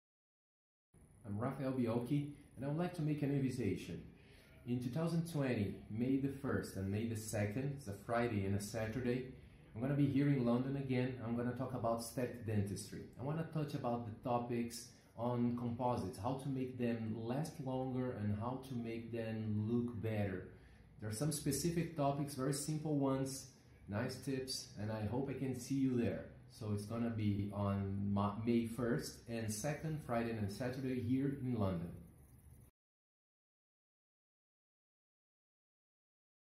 Rafael Beolchi - See me at the Dental Innovation Symposium 2020
You will be able to discuss available techniques with like minded individuals and discover advanced aesthetic procedures with dental composites. In this session you will be able to identify which materials would be able to reproduce even the smallest anatomical and optical details of the natural tooth.

Join Rafael at the Dental Innovation Symposium 2020, 1st - 2nd May, London.